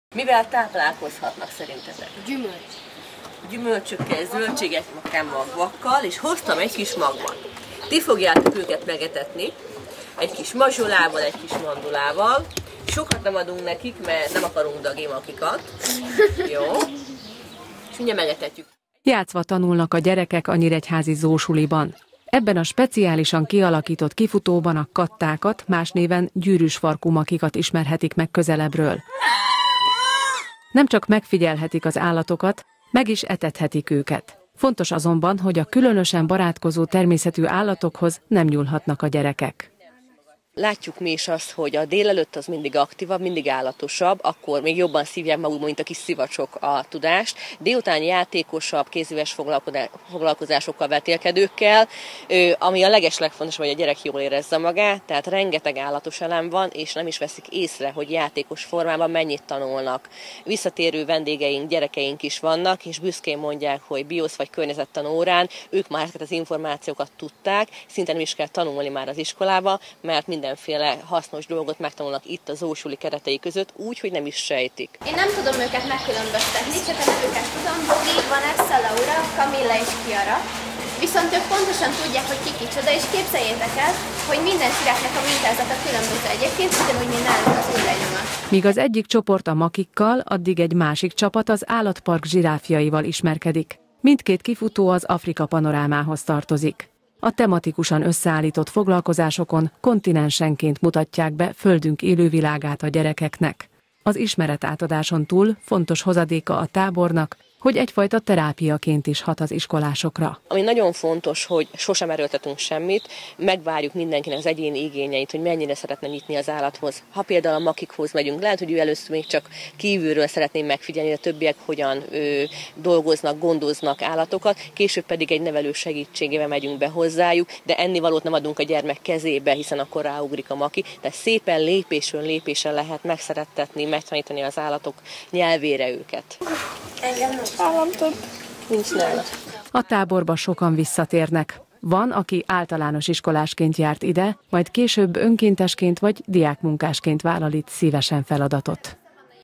ZOO-Suli
Egzotikus állatokat etethetnek, horgászhatnak és tevegelhetnek. Izgalmasan telnek a Zoo-Sulisok hétköznapjai a Nyíregyházi Állatparkban. A gyerekek egy hét alatt tematikus foglalkozásokon keresztül ismerkednek Földünk élővilágával.